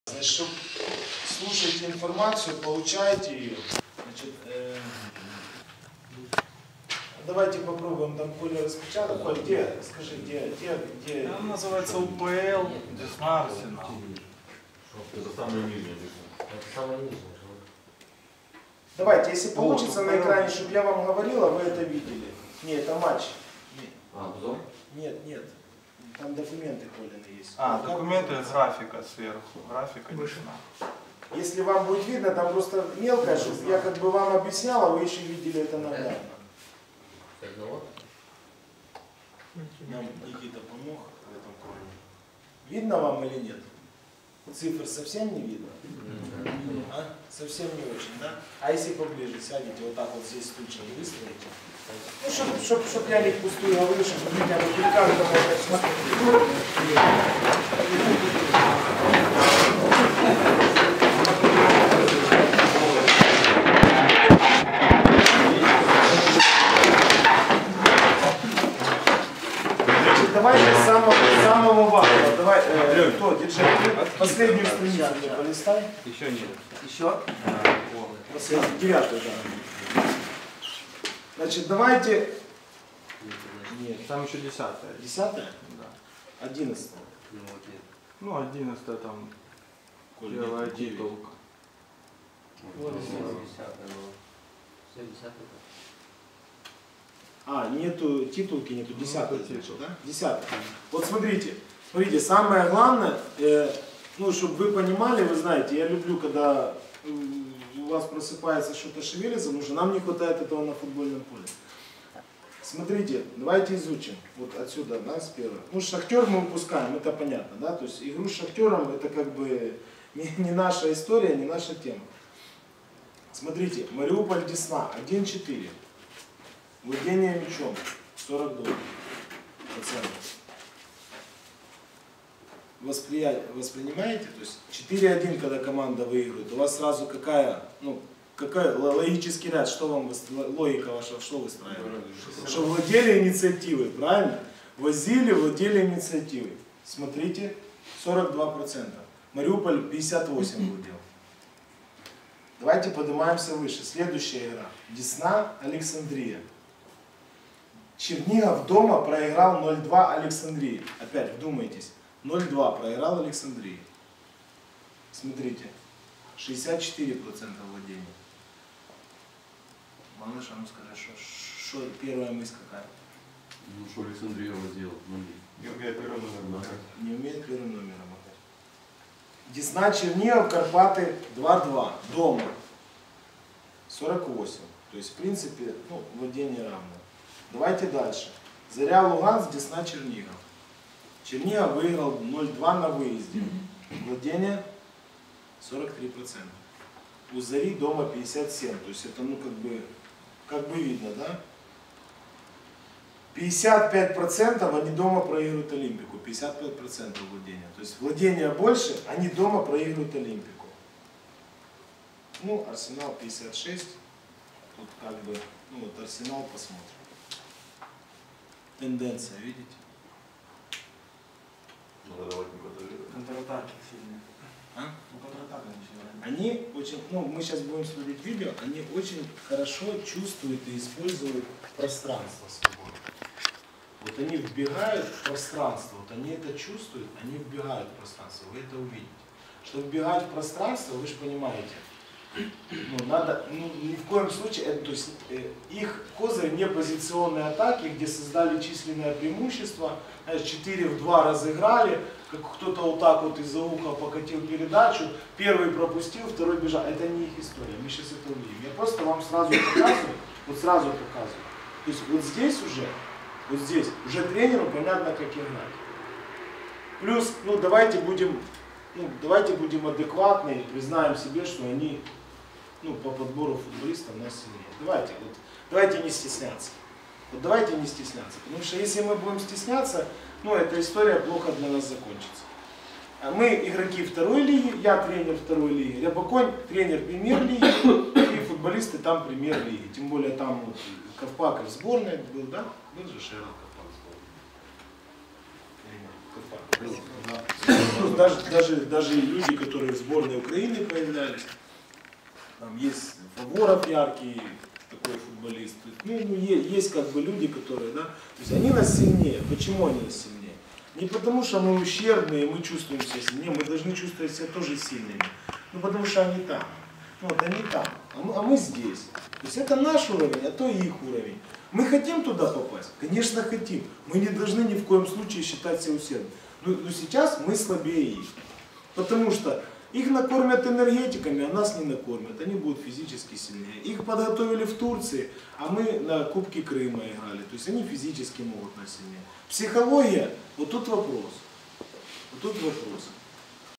Кристалл - Десна. Установка Сергея Шевцова за день до матча (25.09.2018)
В сентябре 2018 года я побывал на тренерской установке Сергея Шевцова и собственными глазами увидел, как футболисты херсонского "Кристалла" готовятся дать бой черниговской "Десне" в матче Кубка Украины.